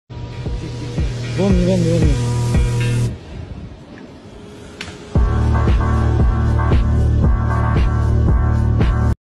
a Ukrainian soldier firing a Stinger MANPADS at a Russian aircraft.
Russia-Ukraine War: Reportedly video of a Ukrainian soldier firing a Stinger MANPADS at a Russian aircraft. The source claims a Su-34.